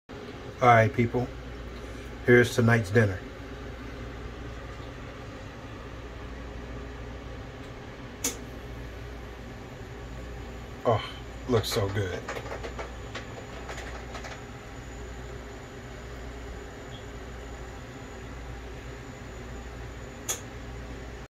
I made cheap and easy Ramen for dinner!
I made some ramen with trhe instant noodles, but I did not use the flavor packet!!